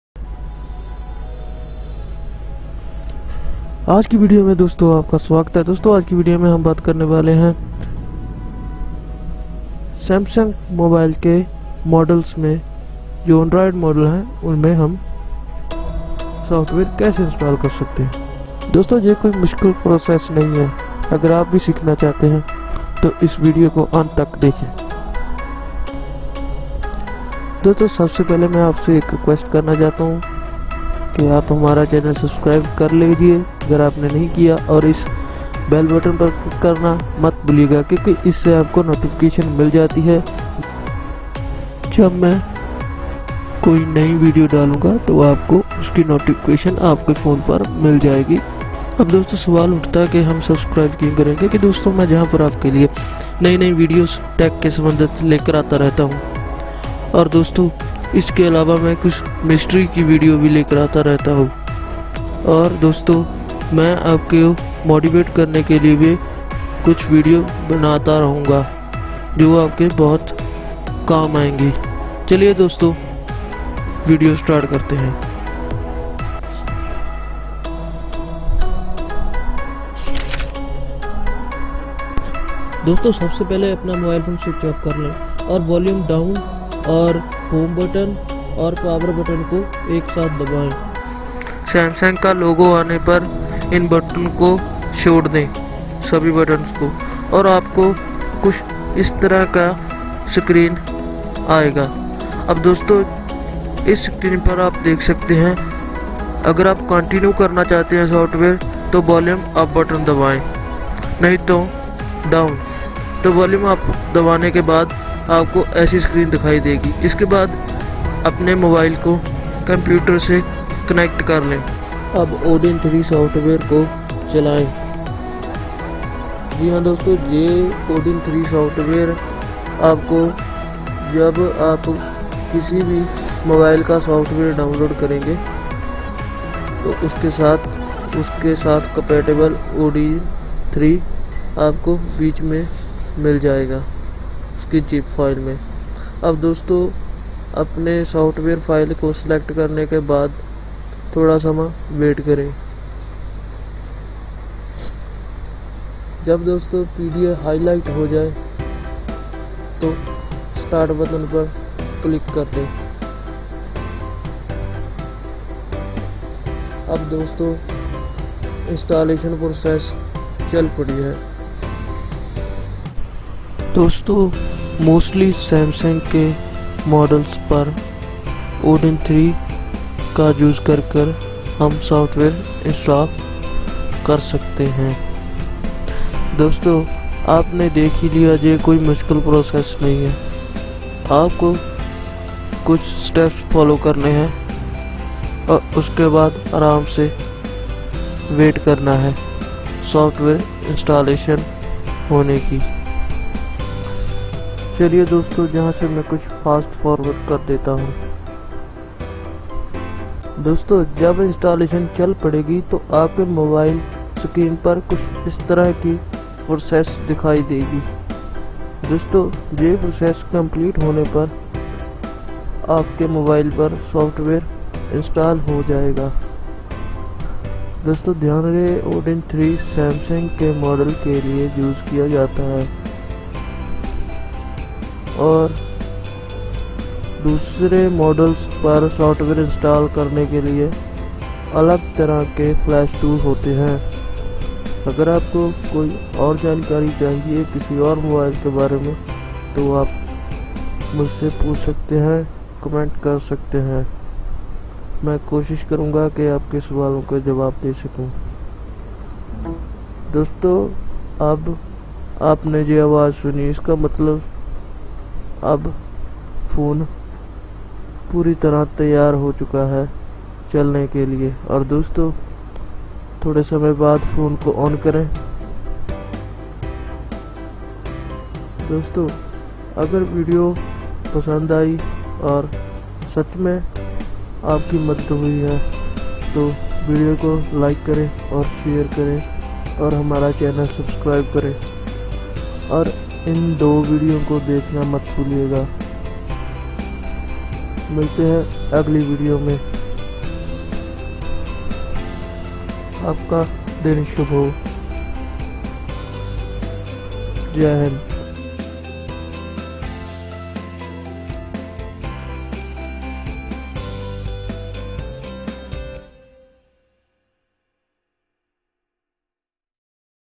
How do reinstall the software on my samsung phone | in Hindi
How To Install Official Android on Samsung Galaxy all models in Hindi

Made by real information

This does not require a rooted device, it will work on any android device!

This video will show you a how to install the latest firmware on your Samsung Galaxy all models. This is an official build of android any for the Samsung Galaxy all that has been leaked, it is supposed to be the test firmware.

Be sure to back up before you do this, your data and applications will be wiped! Even if they are not it is still suggested to wipe to ensure it is a clean installation.